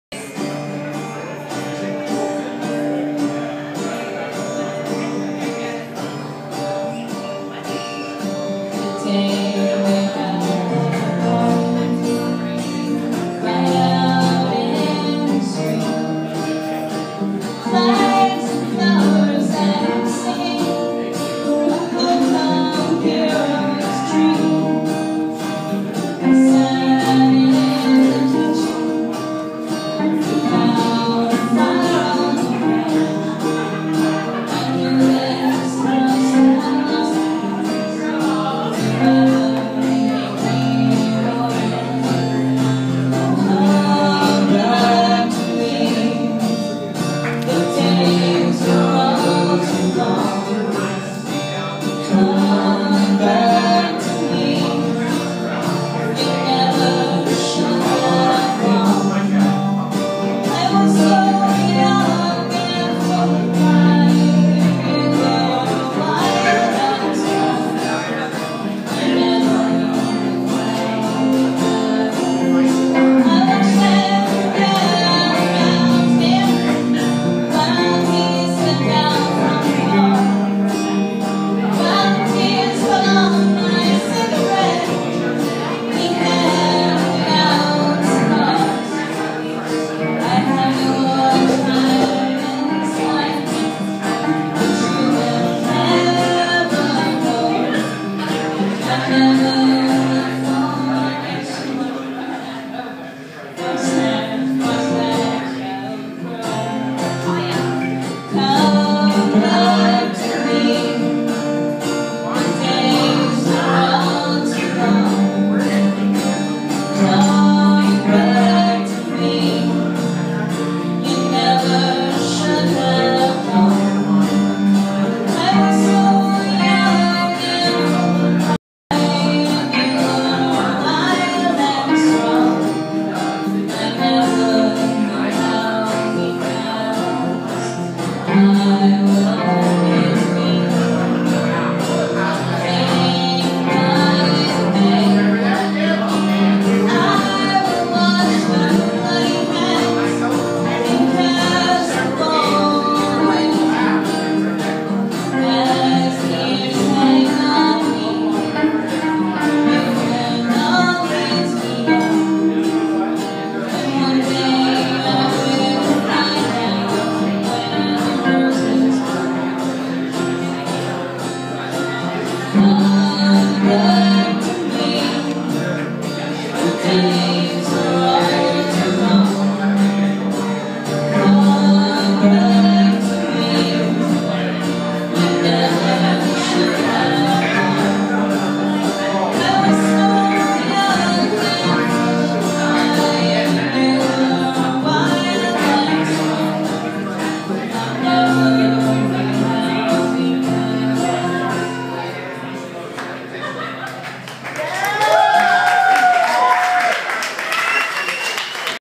Vinnie and Amy at Capitol Garage
Amy's first time rocking the CapG open mic stage